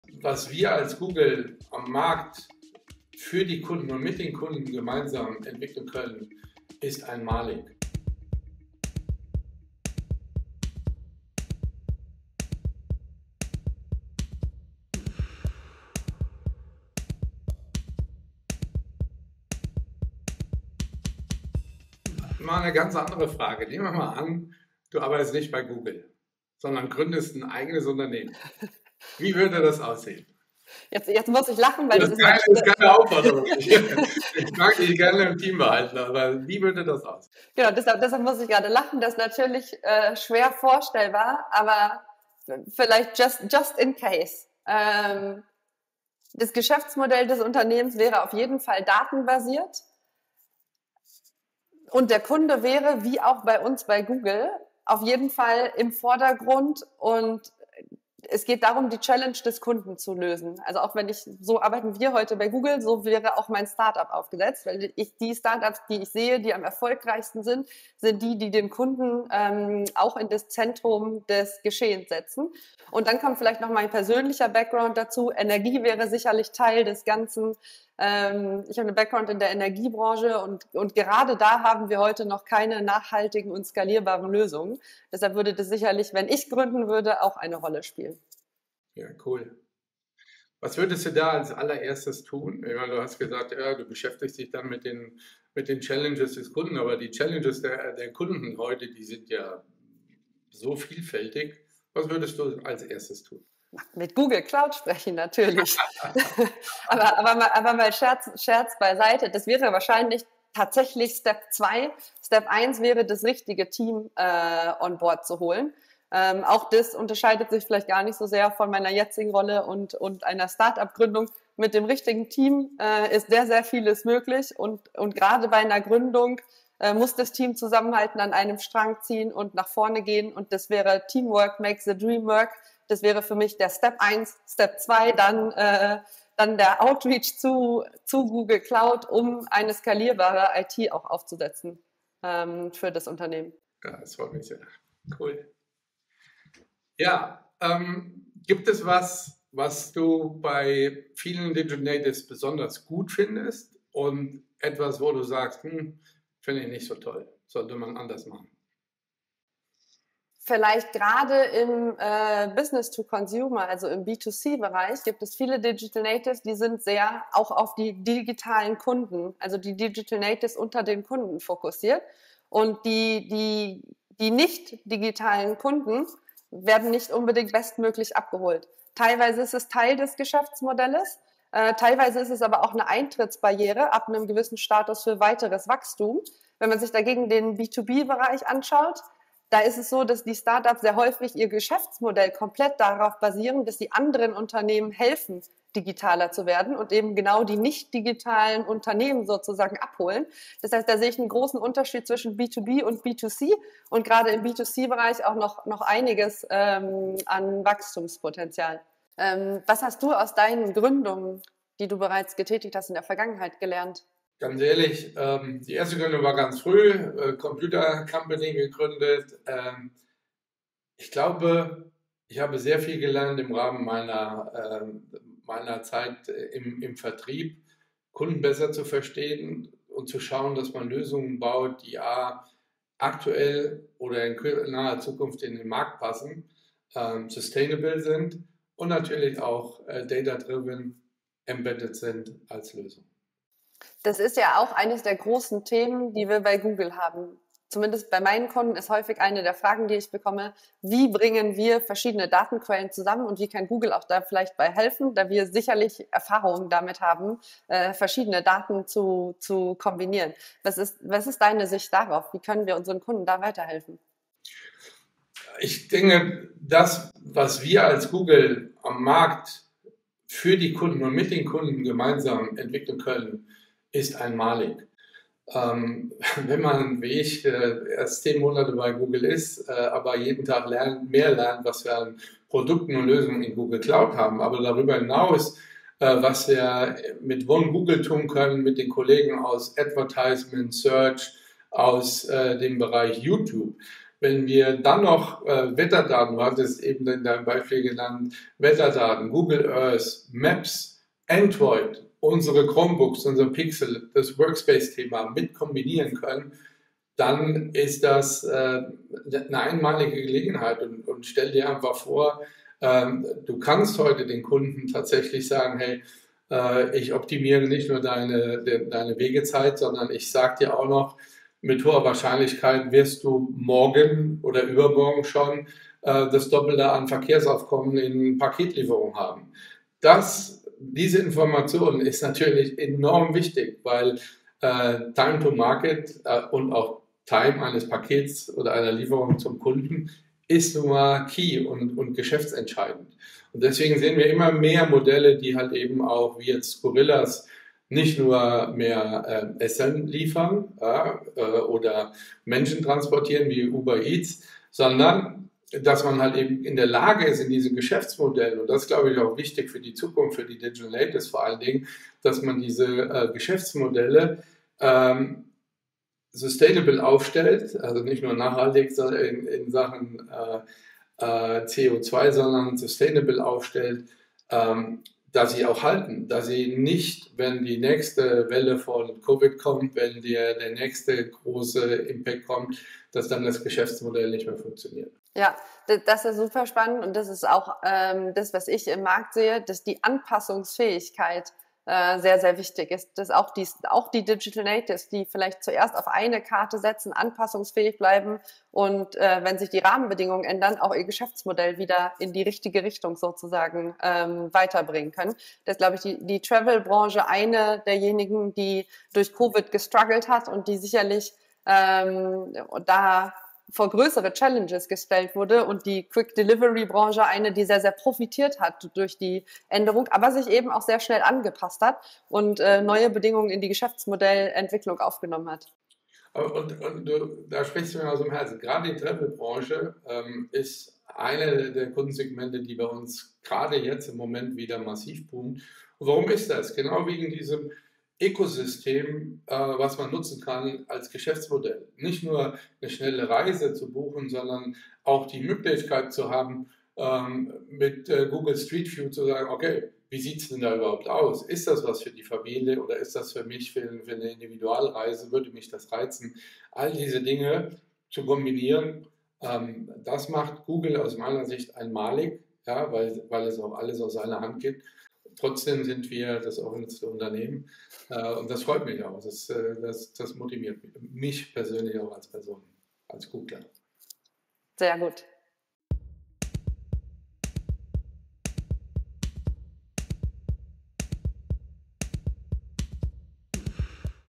Stärken und Schwächen von Digital Natives
In welchen Branchen sind Digital-Native-Unternehmen erfolgreich? In welchen Bereichen haben sie zu kämpfen? Bernie Wagner, Google Cloud MD Deutschland, gibt Antworten.